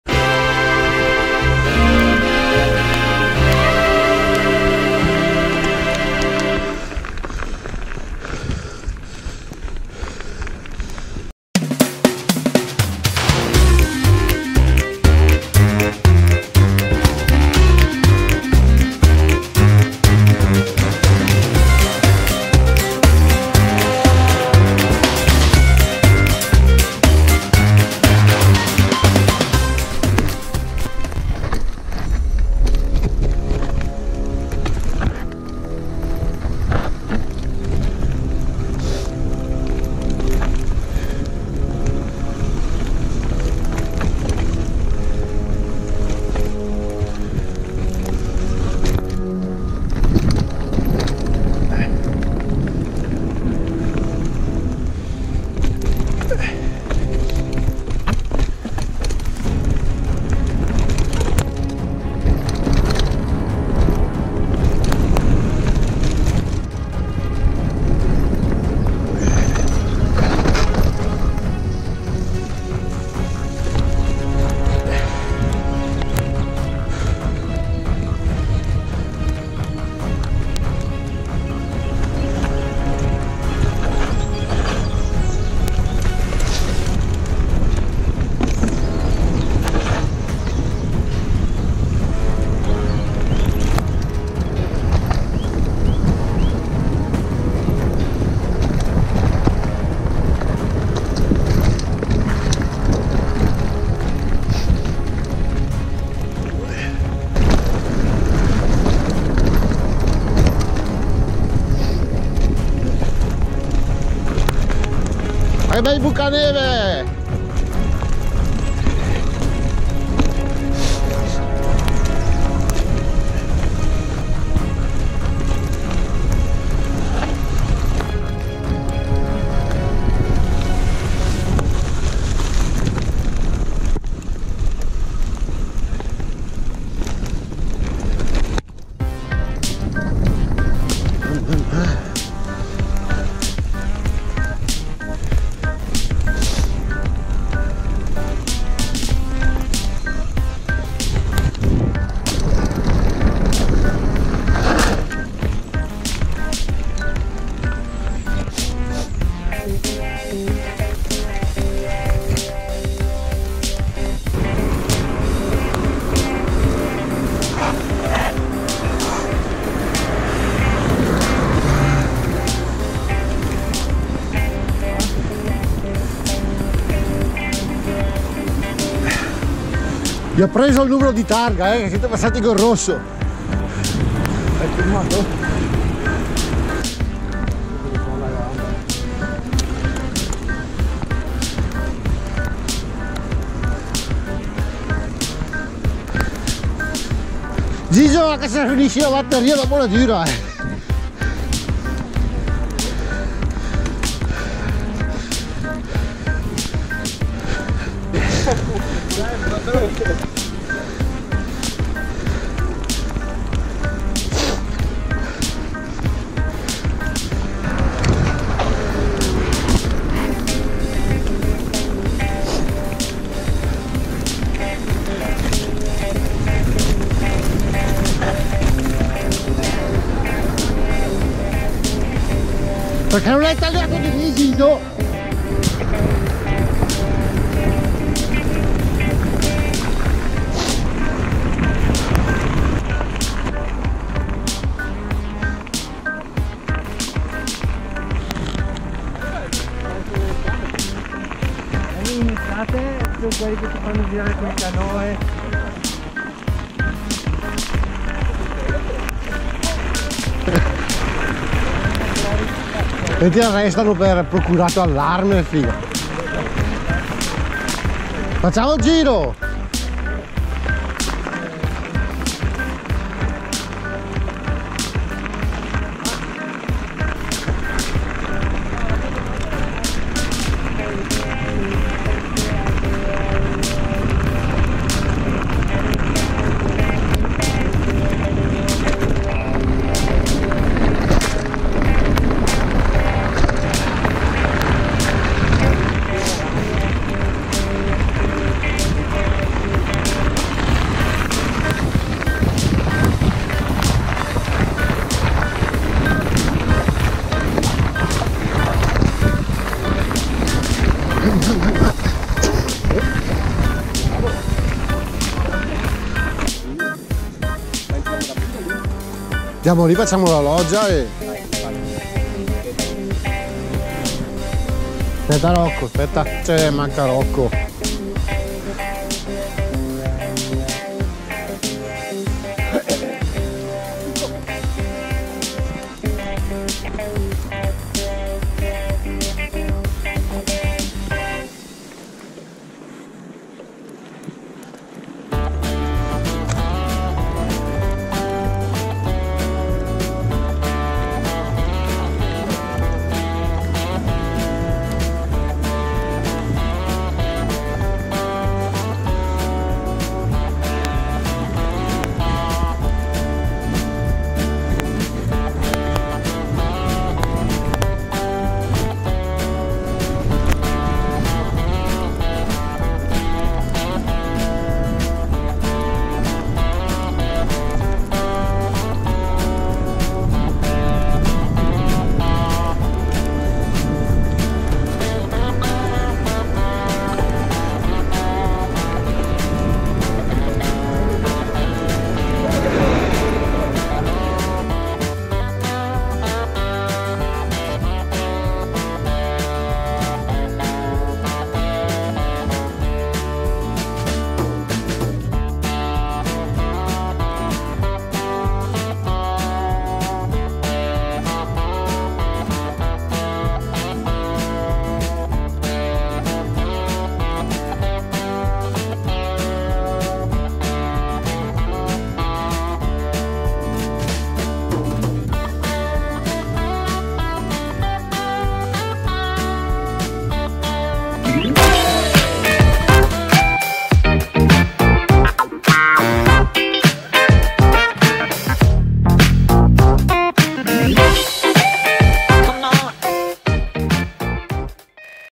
giro cittadino
Partiti alle 7 io e l'amico gigio abbiamo fatto il colle san Giuseppe e scesi dalla val fredda..per poi unirci a Rocco Manuel e Alberto per un giro cittadino…...con aperitivo in piazza duomo